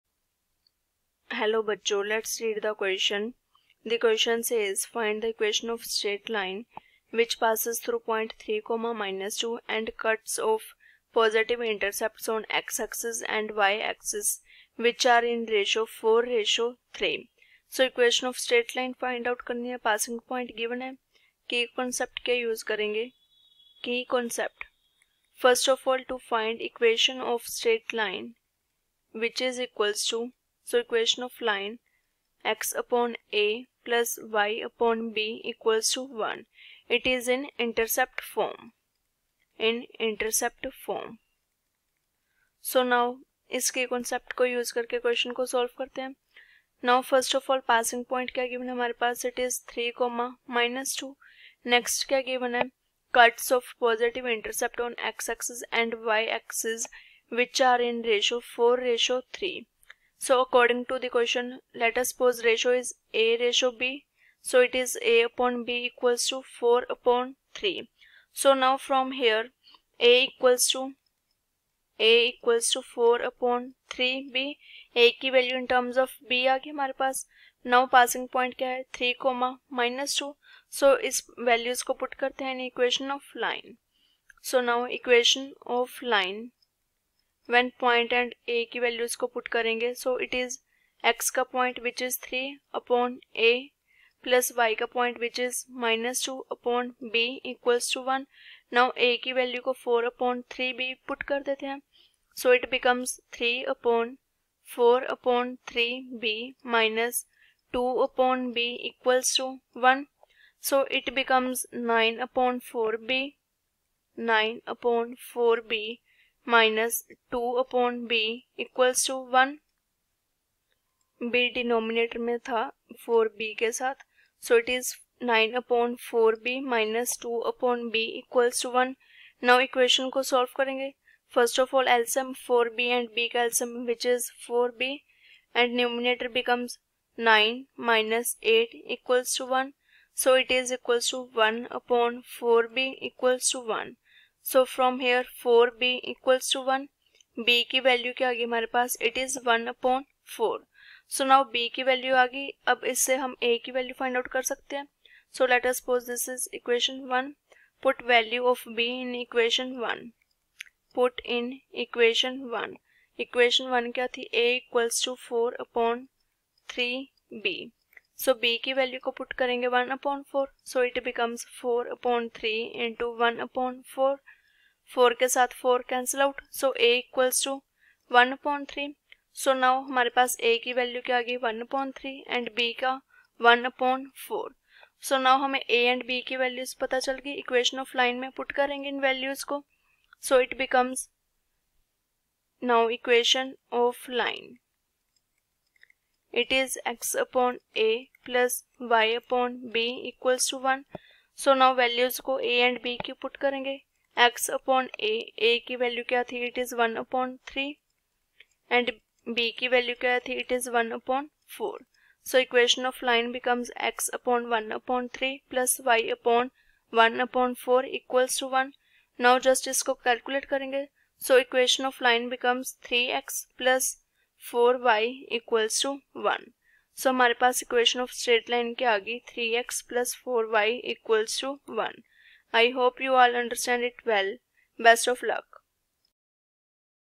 Find the equation of the straight line which passes through the point \( (3,-2) \) and cuts off ...
Find the equation of the straight line which passes through the point \( (3,-2) \) and cuts off positive intercepts on the \( x \)-axis and \( y \)-axis which are in the ratio \( 4: 3 \).